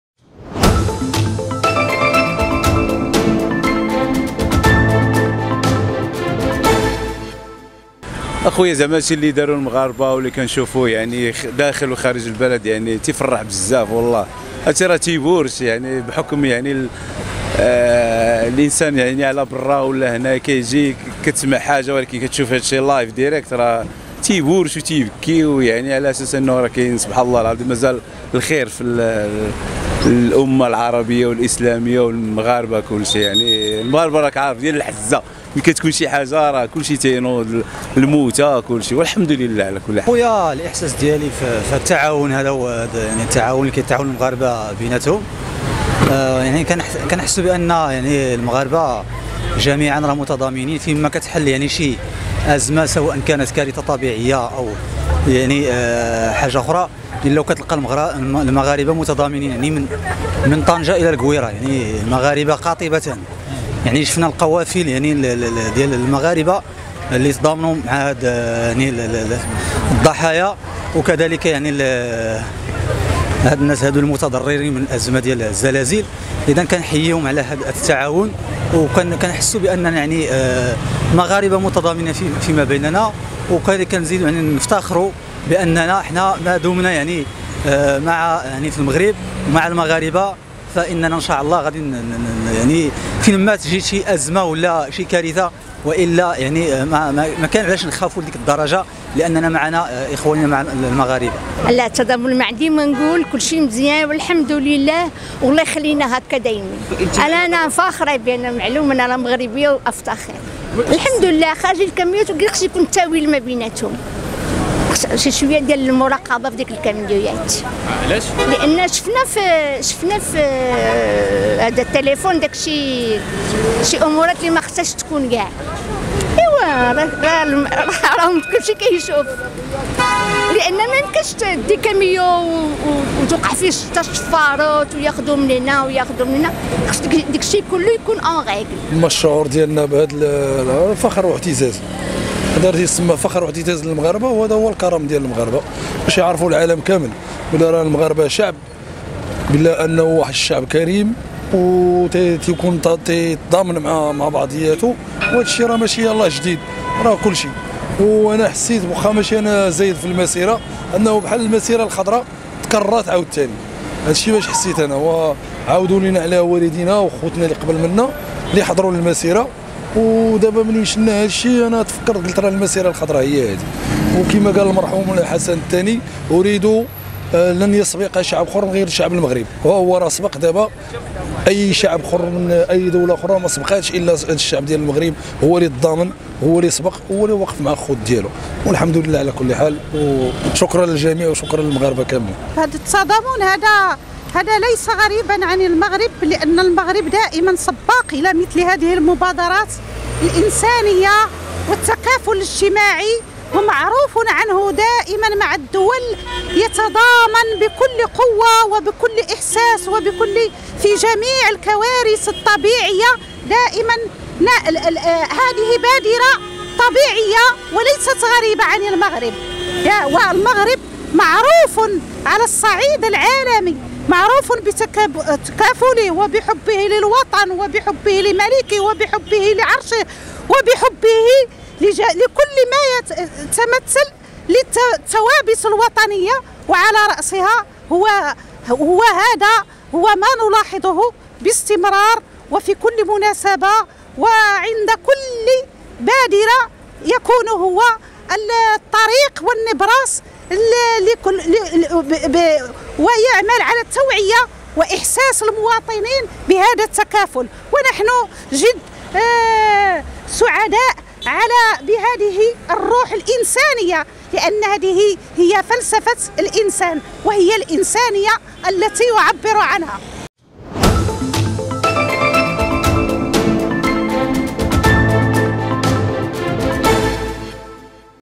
"هادشي كيفرح بزاف"..تعرف على ارتسامات المواطنين حول مساعدات الشعب لضحايا زلزال الحوز
عبر مجموعة من المواطنين عن سعادتهم الكبيرة بخصوص التضامن الشعبي مع ضحايا زلزال الحوز، التي بينت أن المغاربة دائما يتوحدون في  عمل الخير من طنجة لكويرة، واعتبر البعض أن ما يحدث يشبه المسيرة الخضراء، حيث توحد المغاربة فيما بينهم يدا واحدة.